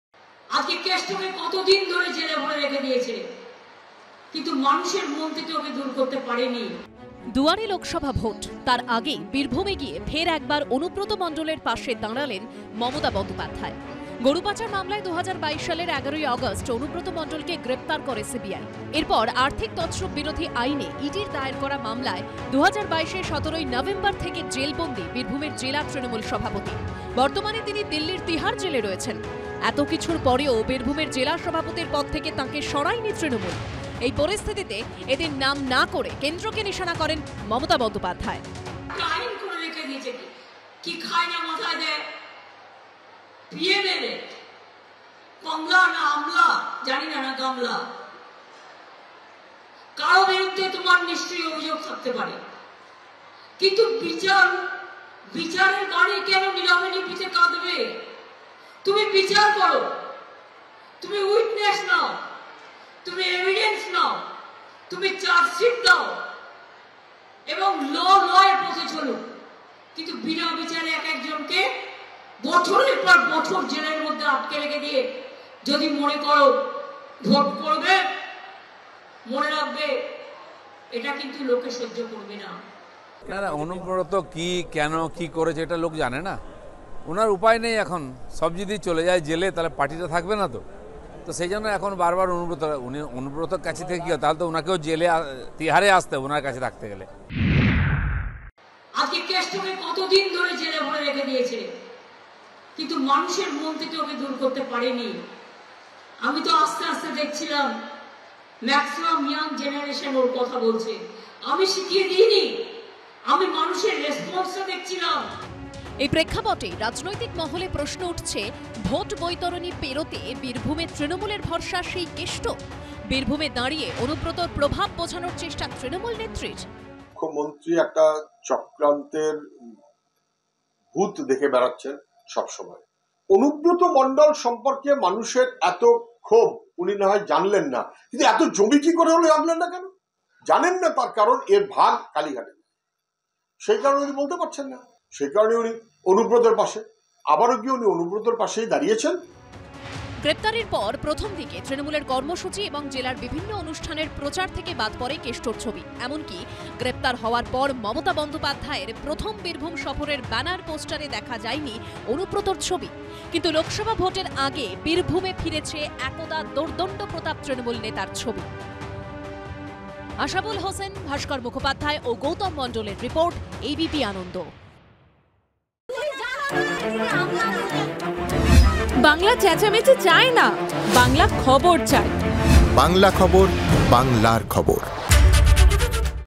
Mamata Banerjee: 'মানুষের মন থেকে দূর করতে পারেনি', অনুব্রতের পাশে দাঁড়ালেন মমতা। ABP Ananda Live
Anubrata Mondal: 'মানুষের মন থেকে ওকে দূর করতে পারেনি', ফের অনুব্রতের (Anubrata Mondal) পাশে দাঁড়ালেন মমতা (Mamata Banerjee)। গরুপাচার মামলায় গ্রেফতার করে হয়েছিল অনুব্রতকে।  তারপর থেকে বিভিন্ন সময় অনুব্রতের স্তুতি শোনা গেছে মমতার মুখে।  এদিন ফের একবার খোলাখুলি অনুব্রতের পাশে দাঁড়ান তিনি। ABP Ananda Live